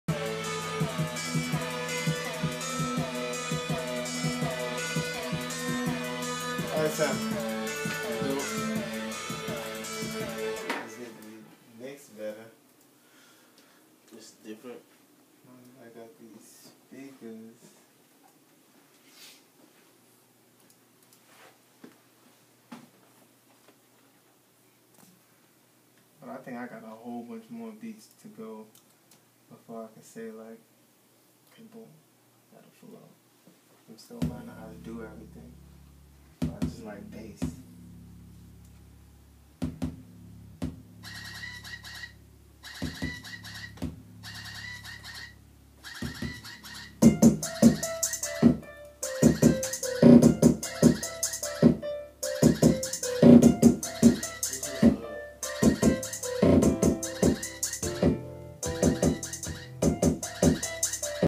Juno Bourne Live Studio session
slpgtv.com - Sitting in the studio with Artist/Producer Juno Bourne as he goes through tracks he's working on.